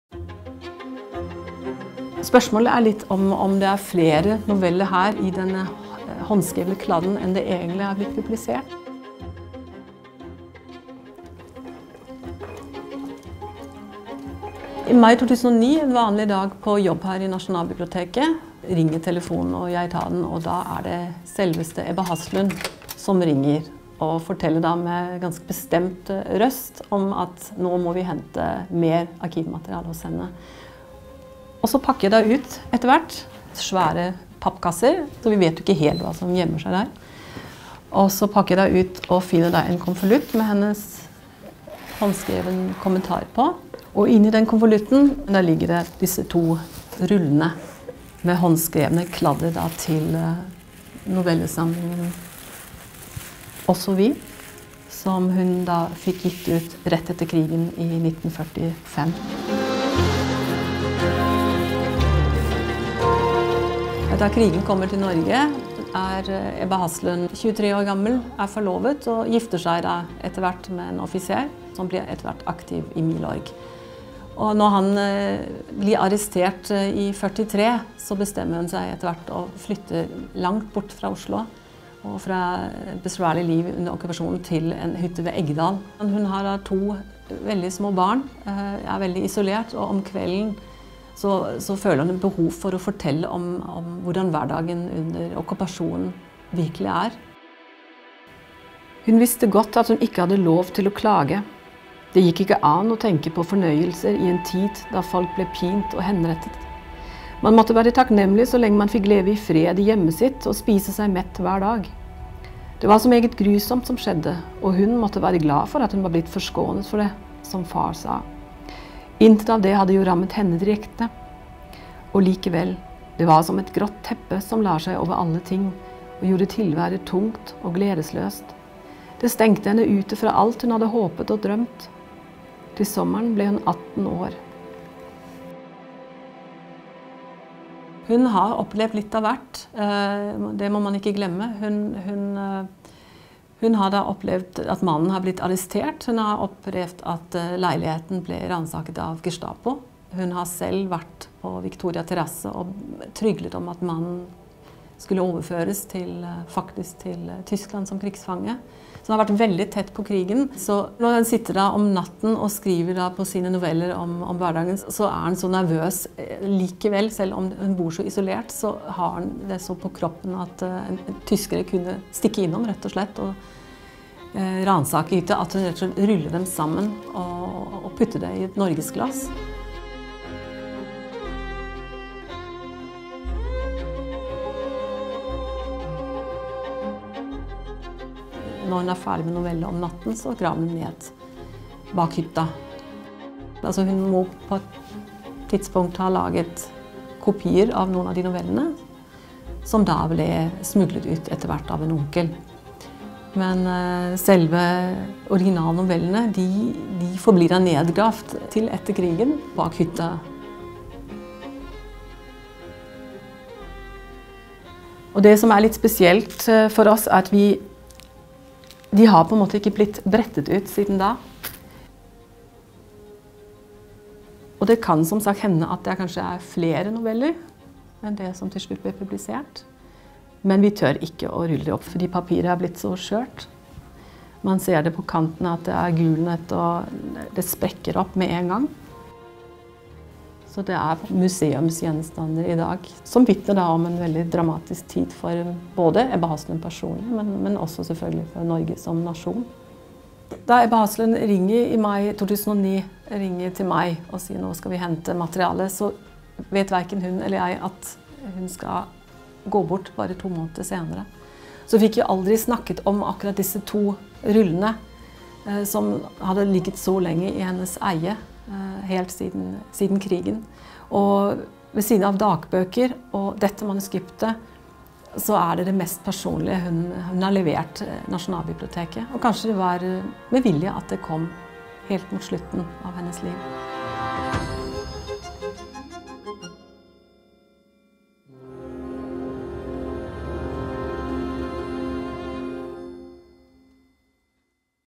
Finnes det upubliserte Ebba Haslund-noveller her?
Manuskriptene lå nedgravd bak hytta, gjemt for nazistene.

Kort tid før Ebba Haslund gikk bort, ringte hun Nasjonalbiblioteket om en helt spesiell donasjon. Manuskriptene til hennes debutbok var blitt skrevet under krigen og holdt skjult for nazistene. Nå var hun klar for å gi dem fra seg.

Men hva befinner det seg egentlig på rullene? Kan det være upubliserte noveller her?